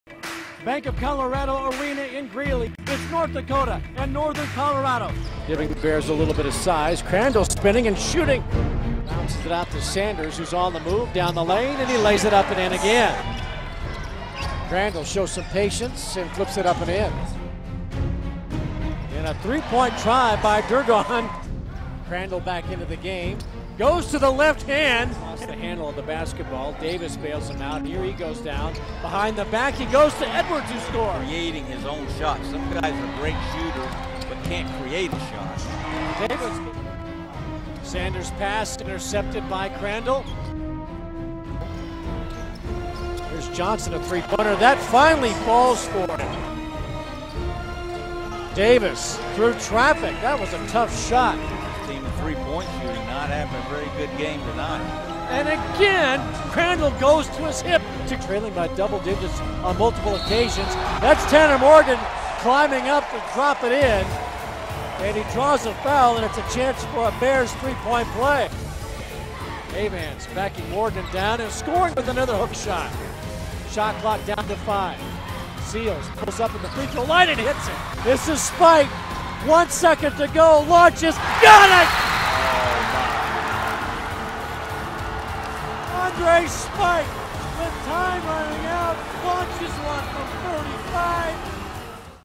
UNC Bears Men's Basketball vs. UND Highlights
The Bears defeat North Dakota in an overtime thriller.